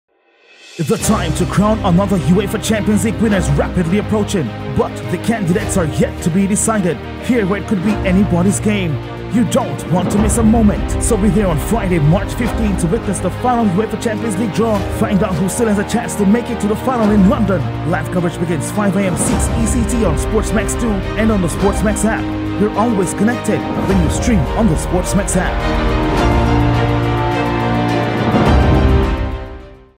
The UEFA Champion League | Fri. Mar. 15 | Quarter-Final & Semi-Final Draws | on SportsMax2 and App!
The UEFA Champion League | Fri. Mar. 15 | Quarter-Final & Semi-Final Draws | on SportsMax2 and SportsMax App!